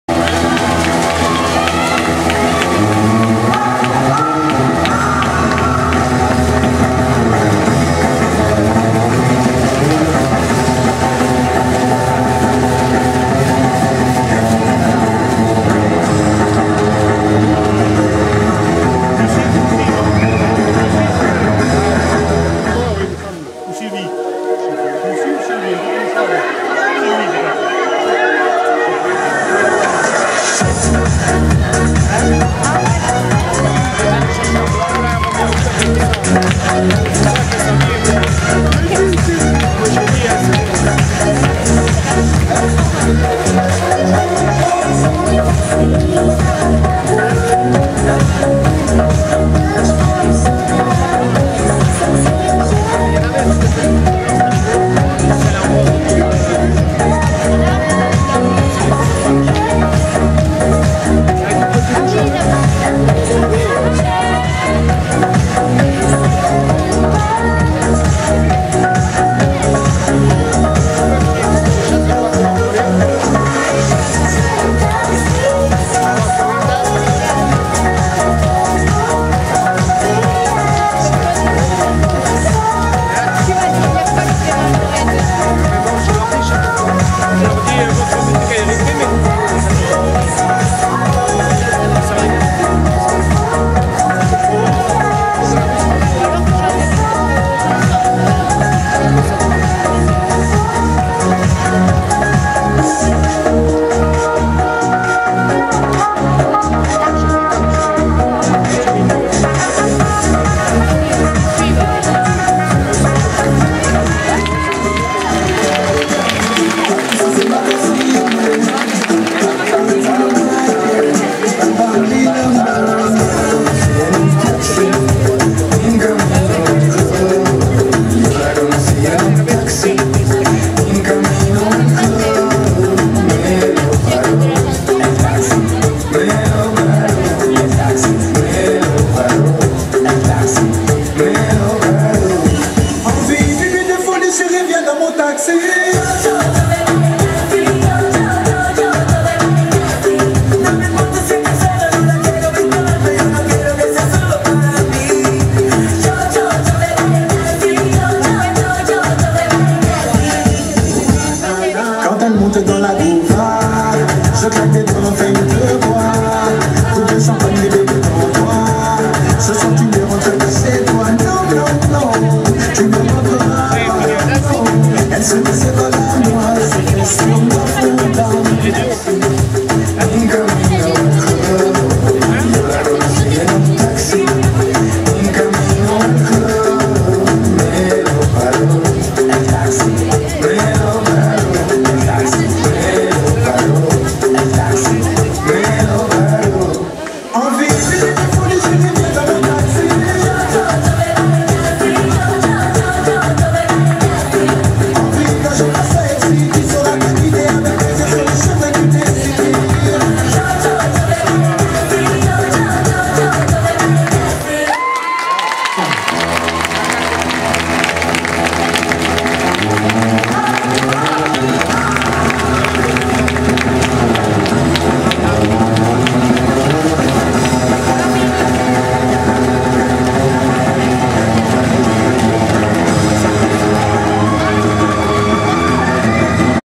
Les Gagagirls chorégraphie moyennes à pompons.
Danse sur le thème des taxis, chorégraphie pompons moyennes saison 2016/2017